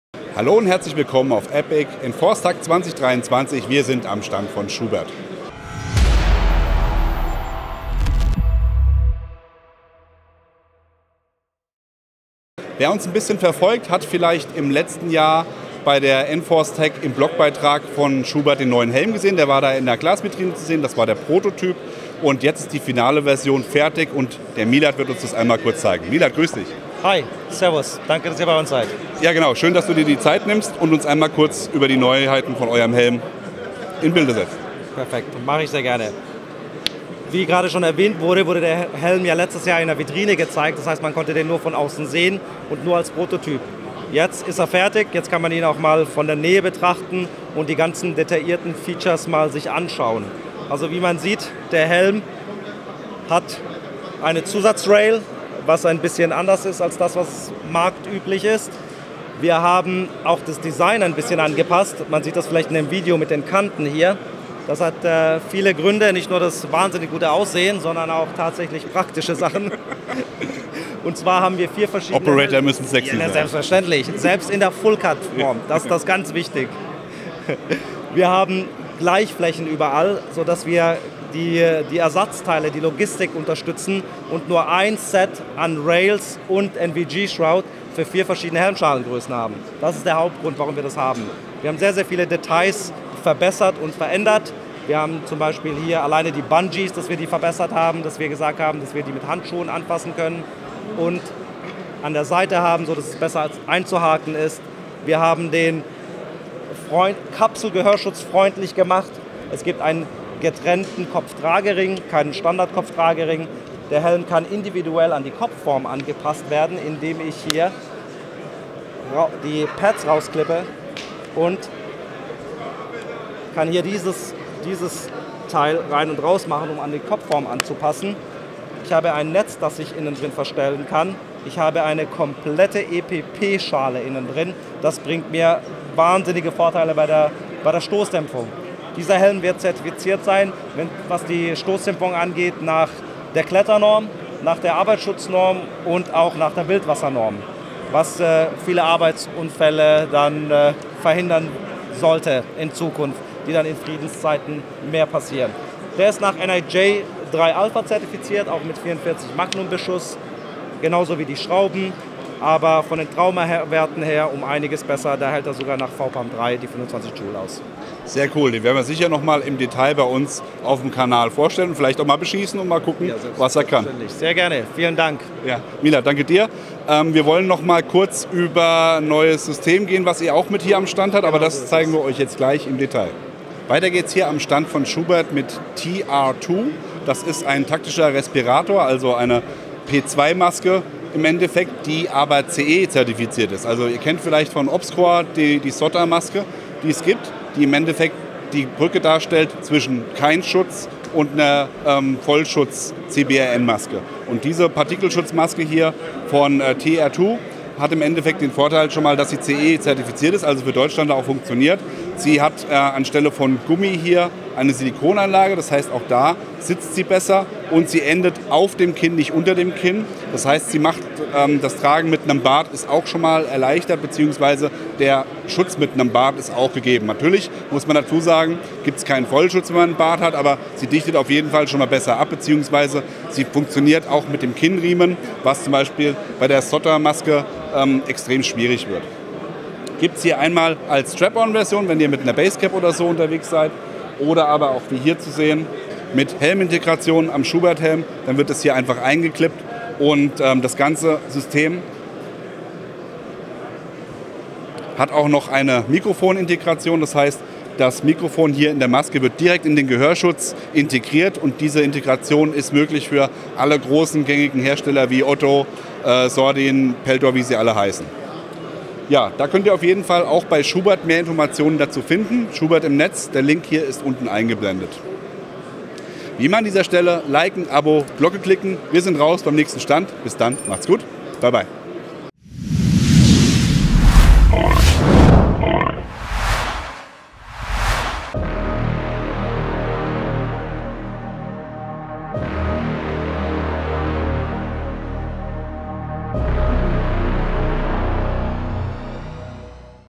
ENFORCE TAC 2023: SCHUBERTH
Die Firma Schuberth hat letztes Jahr den Prototyp des M100 Helms vorgestellt, dieses Jahr kann man die finale Version anfassen und bestaunen - wir schauen ihn uns im Detail an, dazu kommt noch ein neuer Partner für Partikelfilter!